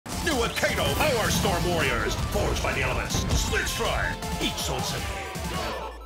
AKEDO | WORLD OF POWERSTORM WARRIORS | 6"
The Legends of Akedo are Mini Battling Figures with Next Level intensity! Choose your Warrior, battle with fury and finish them with a Split Strike! With the All New Powerstorm Series there are 42 new epic Mini Battling Figure toys boosted for Elemental Battle Action!

READY, FIGHT, SPLIT STRIKE!